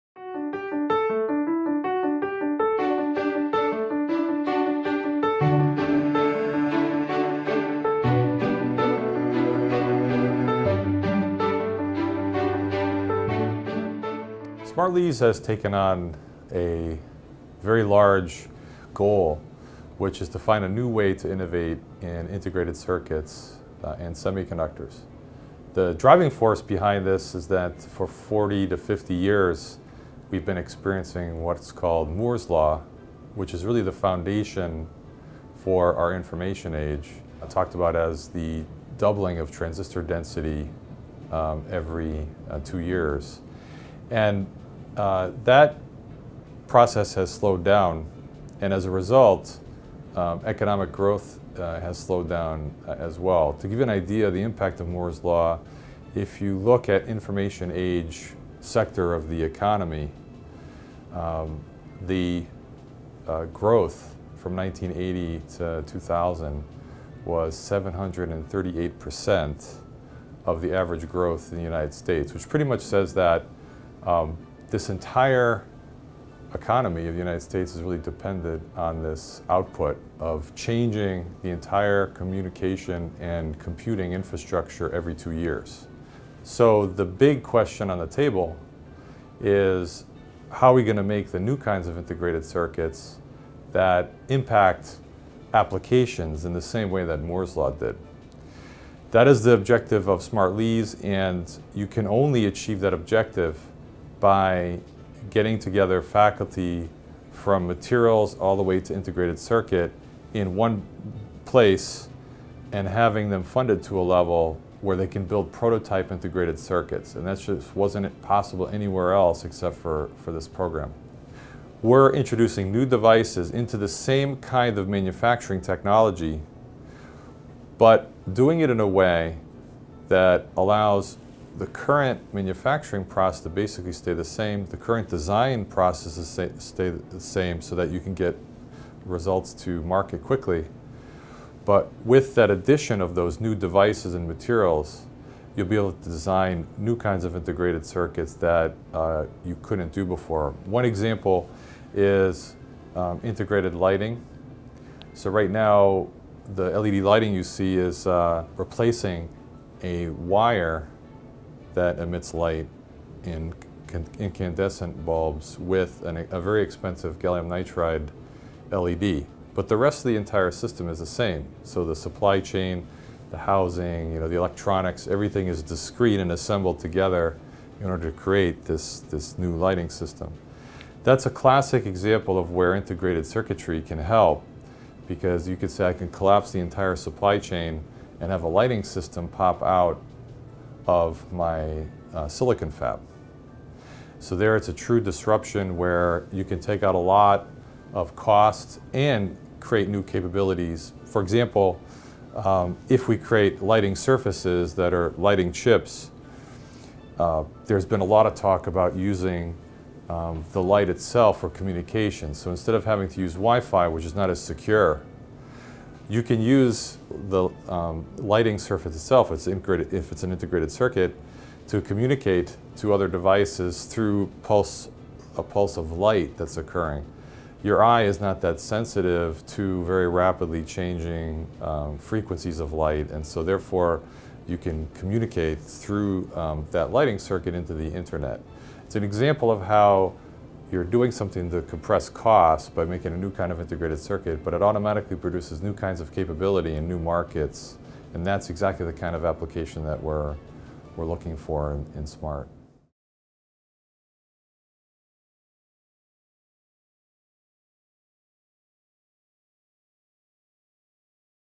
Finding the New Paradigm After Moore's Law
MIT Materials Science & Engineering Professor Eugene Fitzgerald explores new innovation pathways in electronics research.